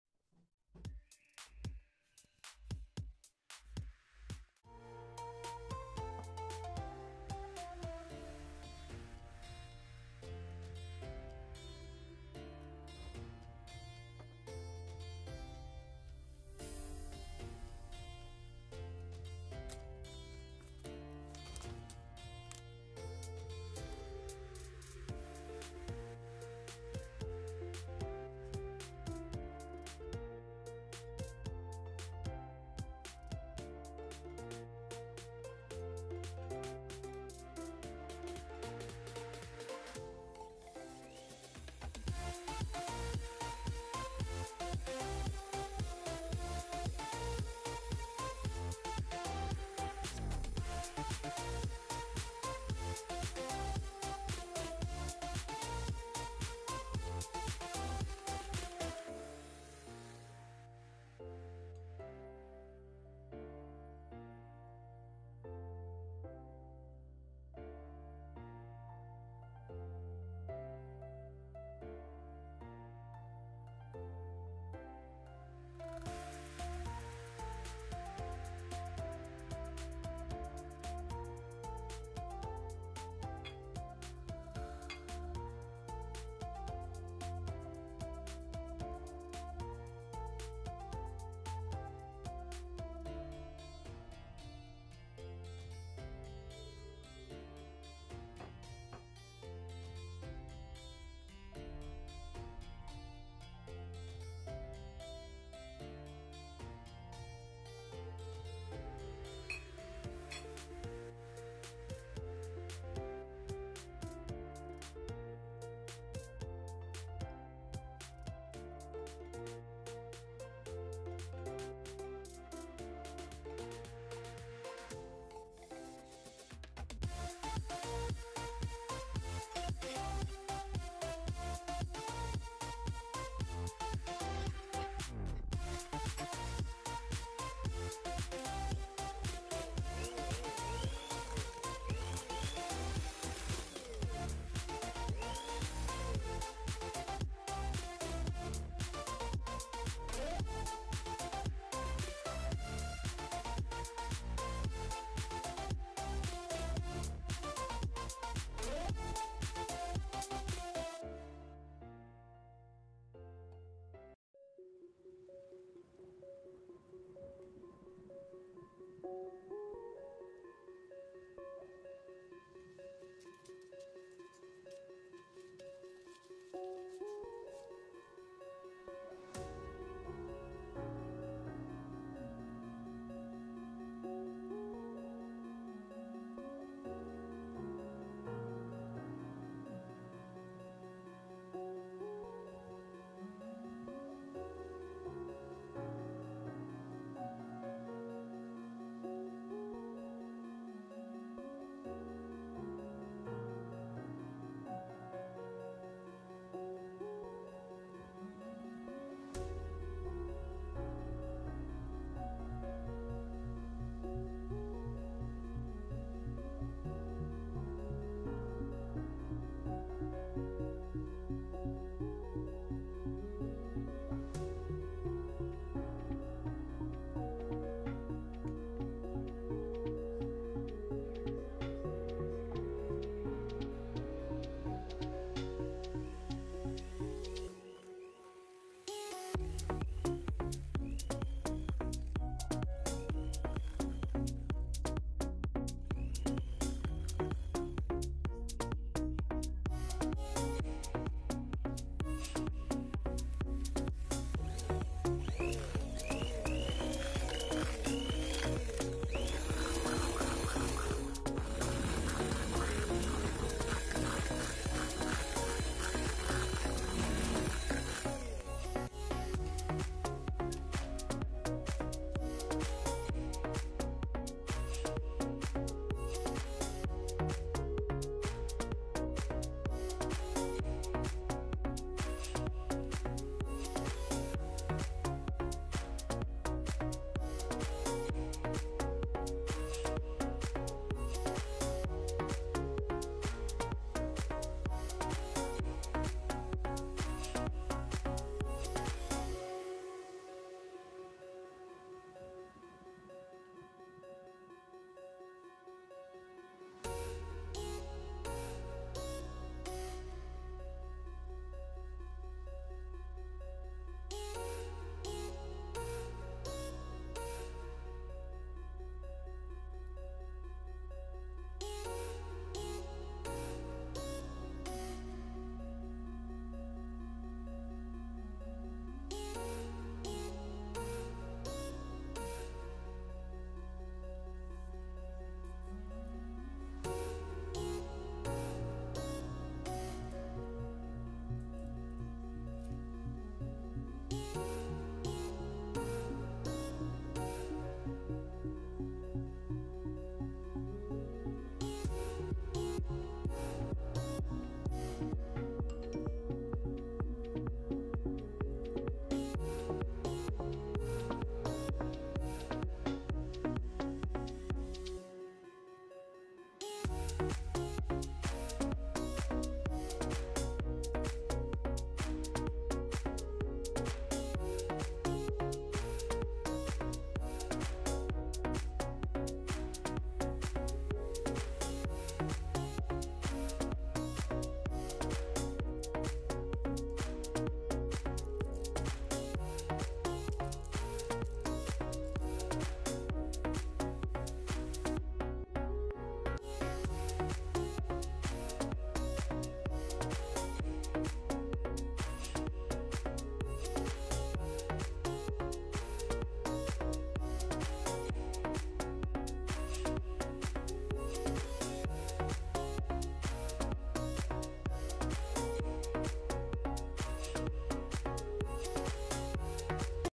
Cox lezetli aĝizda əriyən yerkökü tortu//Морковный торт#tort#морковныйторт#reseptler
Тесто:
Муку 250гр. просеять и смешать с 1.5ч.л разрыхлителем ,1,5ч.л соды ,1,5 корицей и щепоткой соли.
Обжарить 100 гр.орехов в духовке и порубить ножом , не очень мелко.
Чернослив  100 гр.промыть ,просушить нарезать.Морковь 350гр.( 4 средних) натереть.
3 Яйца и один желток взбить с  220гр сахаром до увеличения массы в двое.Влить 180гр растительного масла .Положить следом морковь, орехи,чернослив , цедру одного апельсина , сок апельсина 30 мл.,ваниль размешать и частями добавить муку.Форму диаметром 24см застелить дно пергаментом и смазать маслом.Тесто разделить на две равные части.Печь в предварительно разогретой до 180 ' C  духовке 25-30мин, ориентируйтесь по вашей духовке.

Крем :
Сметану 25% взбить с 100гр.сах. пудрой и 100 гр сгущ.молока , ваниль.